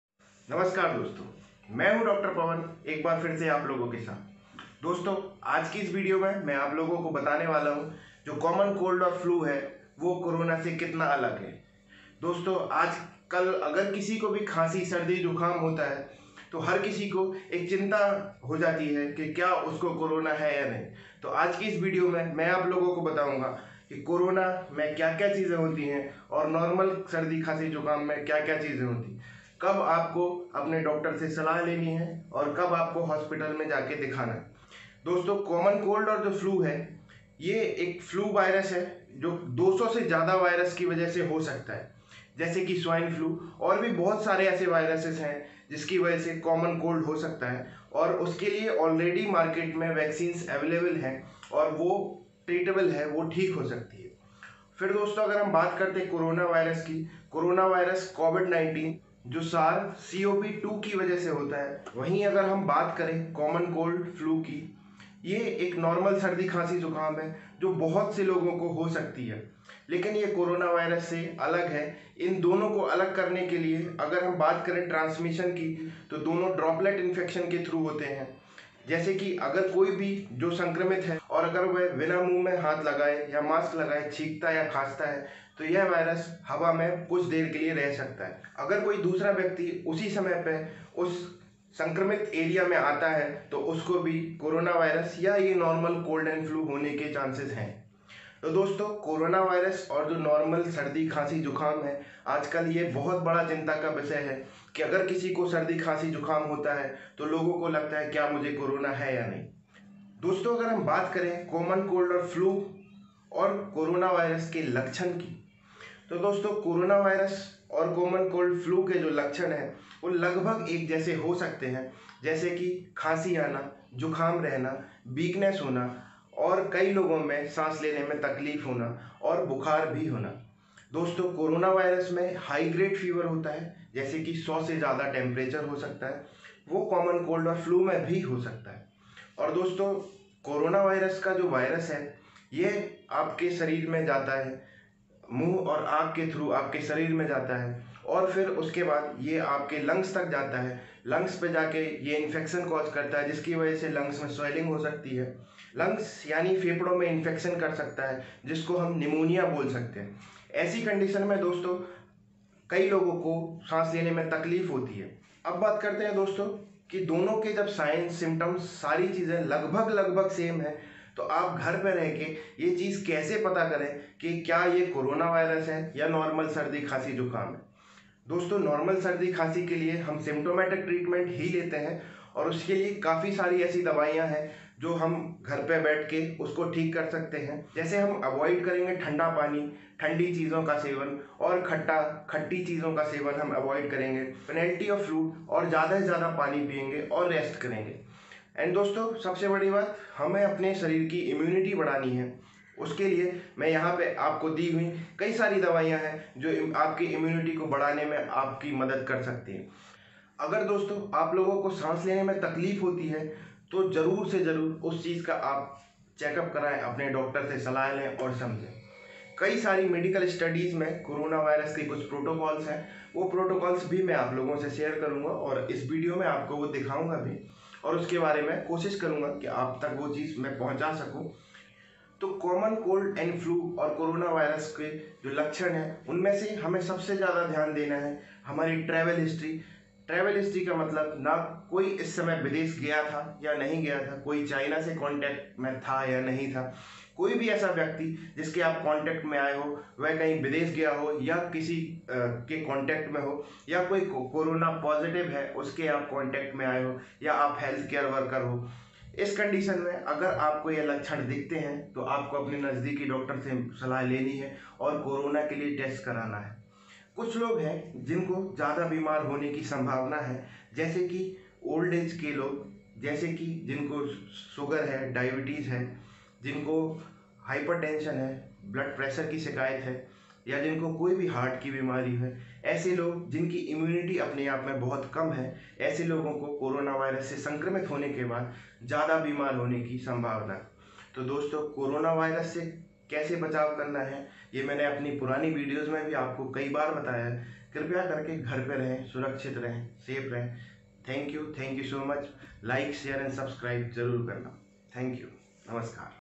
Corona Virus ke Lakshan Vs common Cold/ FLu By. Dr.Pawan Gupta se
Hello Dosto.
My self Dr Pawan Gupta
aaj iss video me apko me bataunga ki
Common Cold aur corona virus ke lakshan me kitna anter hai